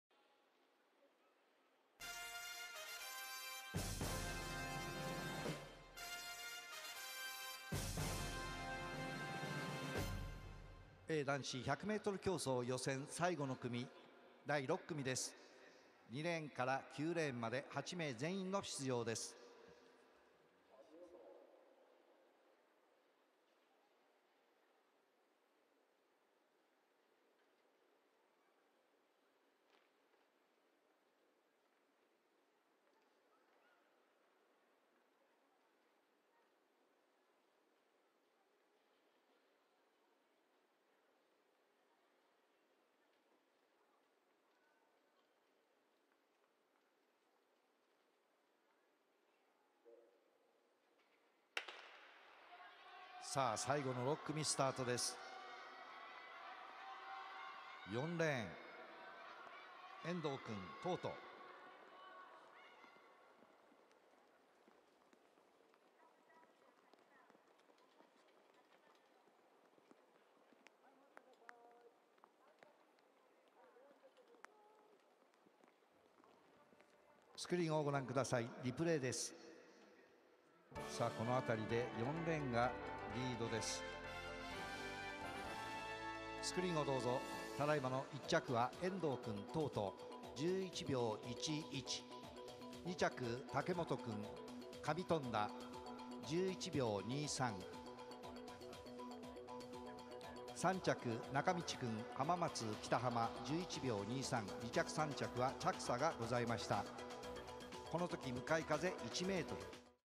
男子A 100m 予選6組 第45回 ジュニアオリンピック
6組 風： -1.0
順位 ﾚｰﾝ ﾅﾝﾊﾞｰ 氏 名  所属団体名 記録
1 4 216 遠藤 慶人 岐 阜 陶都中 11.11
   ｴﾝﾄﾞｳ ｹｲﾄ
2 2 601 竹本 竜也 和歌山 上富田中 11.23
   ﾀｹﾓﾄ ﾀﾂﾔ
3 8 558 中道 泰貴 静 岡 浜松北浜中 11.23
   ﾅｶﾐﾁ ﾀｲｷ
4 3 722 村上 友介 青 森 弘前三中 11.38
   ﾑﾗｶﾐ ﾕｳｽｹ
5 5 1 田島 拓也 千 葉 我孫子白山中 11.53
   ﾀｼﾞﾏ ﾀｸﾔ
6 6 630 藤原 大輝 鳥 取 高草中 11.66
   ﾌｼﾞﾜﾗ ﾀﾞｲｷ
7 9 836 平野 皓陽 愛 知 東海中 11.73
   ﾋﾗﾉ ｺｳﾖｳ
8 7 524 杉山 祐輔 秋 田 横手南中 11.8
   ｽｷﾞﾔﾏ ﾕｳｽｹ

2014年10月31日(金)～11月2日（日）
日産スタジアム（神奈川）